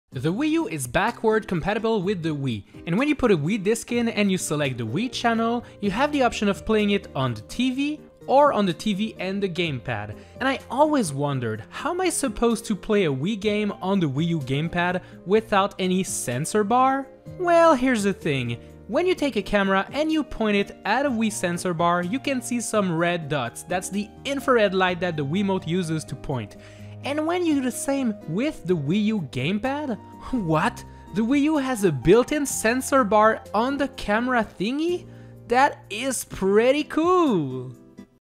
No Sensor Bar on Wii U? No Problem!
You have less than 60 seconds? Well today's is all about the secret sensor bar on the Wii U Gamepad!